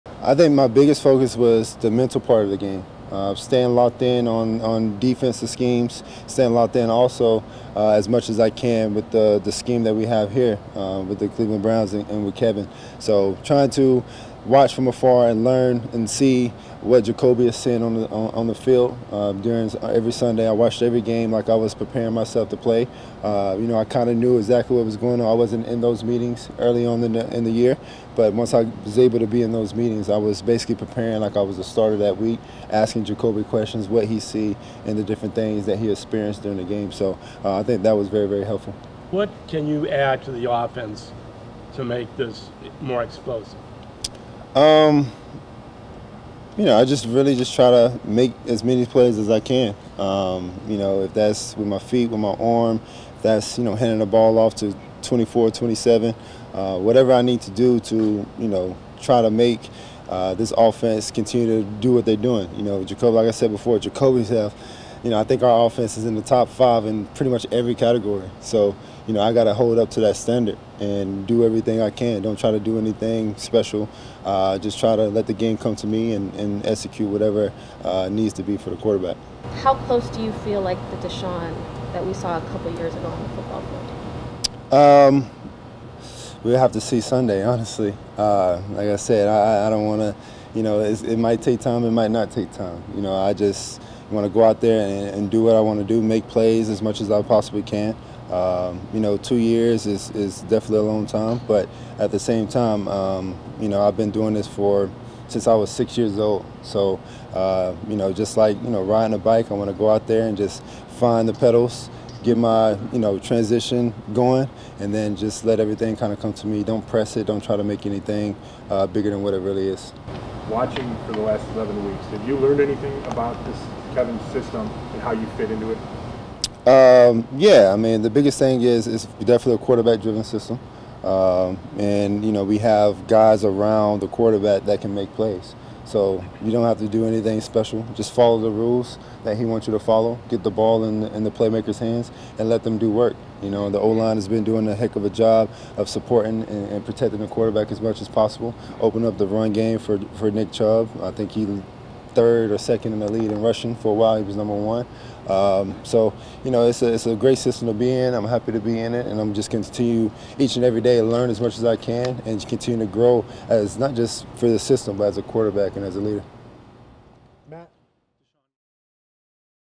Deshaun Watson’s eager to make plays for Browns
Cleveland Browns quarterback Deshaun Watson talks about returning to field as they prepare to face his former team, the Houston Texans, on Sunday at NRG Stadium. This is Watson’s first game back after serving his 11-game suspension for alleged sexual misconduct with 26 massage therapists while in Houston. Thursday, Dec. 1, 2022